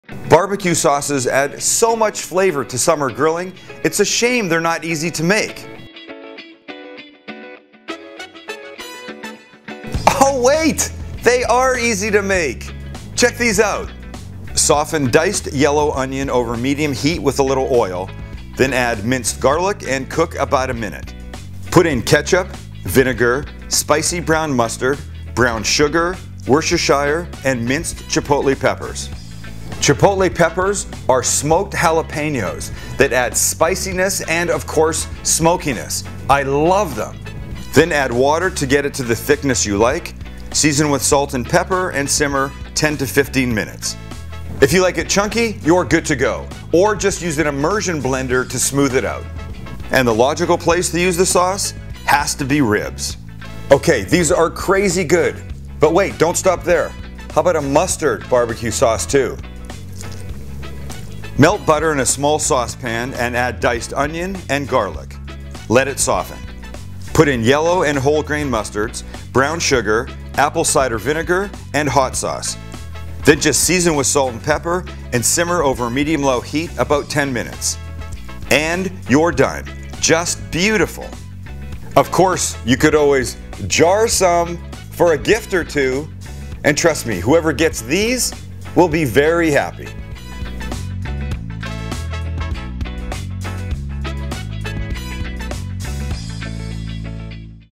Two Terrific Homemade BBQ Sauces
Whether you like your BBQ sauce sweet and spicy or a bit tangy, both of these homemade BBQ sauce recipes are so easy and quick to make and are perfect on ribs.